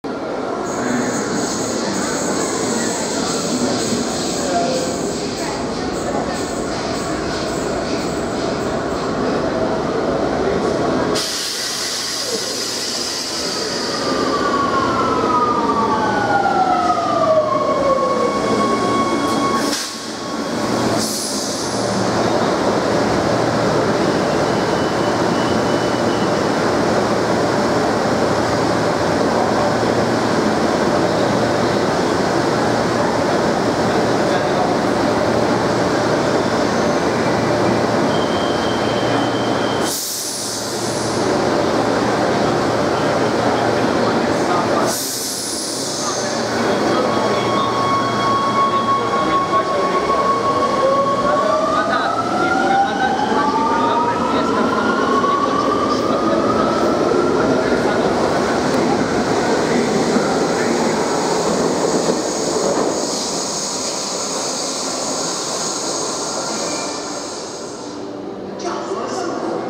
CAF S300 : Arrêt à la station Colosseo sur la ligne B du métro de Rome
Arrêt d'une rame MB100 à la station Colosseo sur la ligne B du métro de Rome .
Métro ligne B : Laurentina - Jonio - Rebibbia .
Réseau ATAC ( Azienda per i Trasporti Autoferrotranviari del Comune di Roma ) .
23 Septembre 2016 .